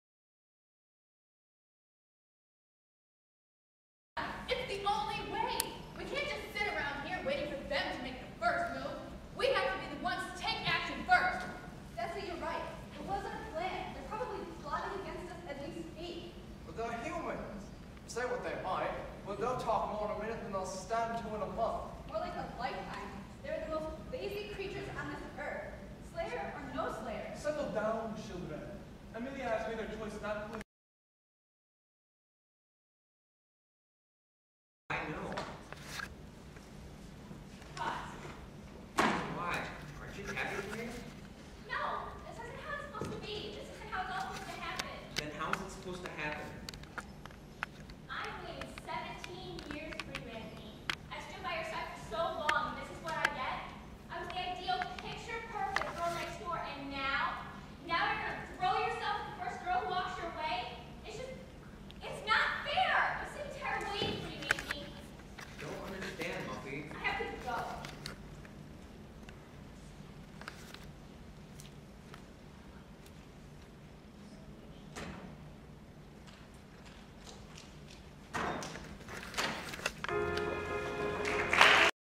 Blood*Crossed Scene IV
The Vampires are in a desperate need for a plan and jacques has decided to grant emilias request to hold back...for now...And Anthony sings of his love for Emilia and it sends muffy in a run for her money. if she can count it.